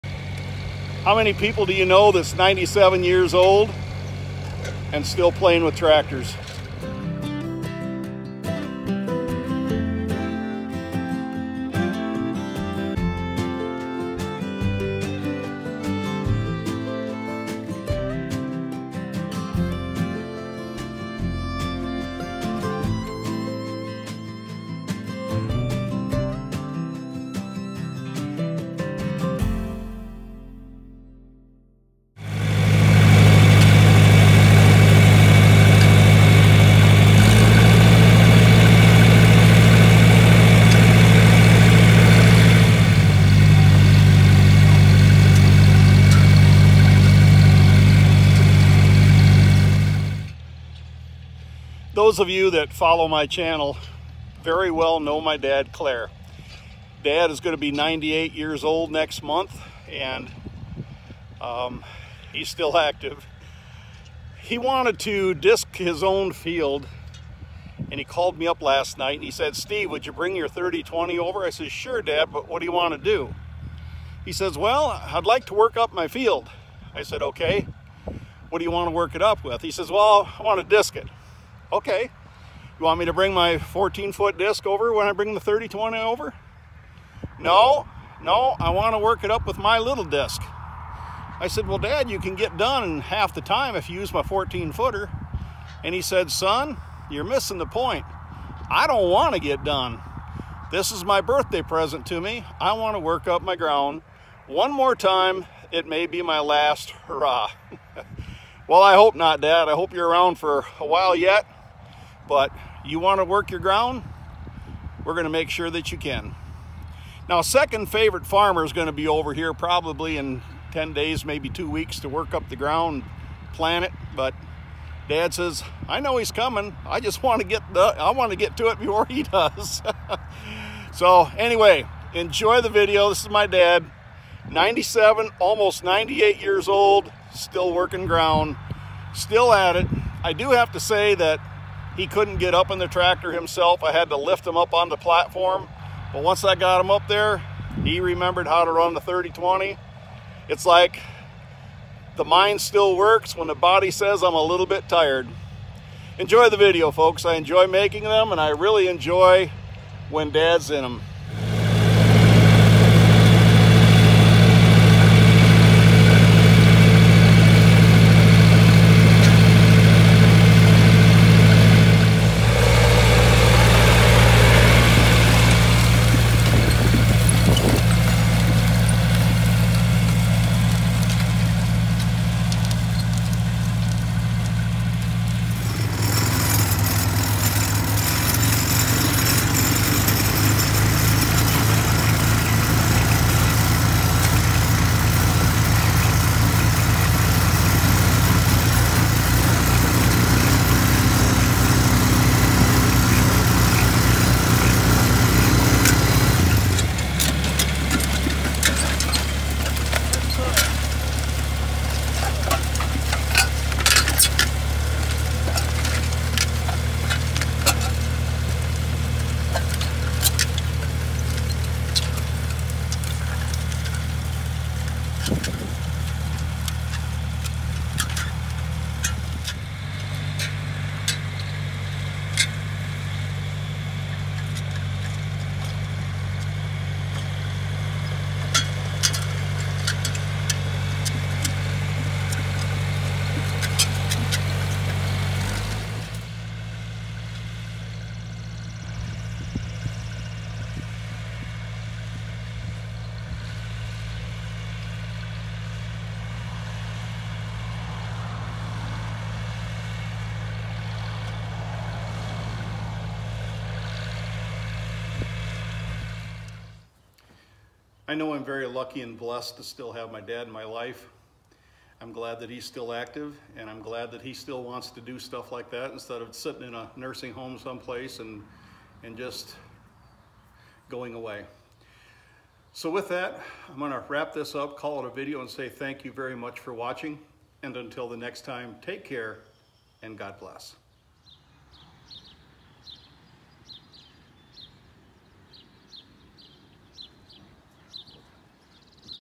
My Dad, 97 years old...still plays with tractors
Dad wanted to work up his ground one last time (he says) so I brought over my 3020 and lifted him up into the operators platform.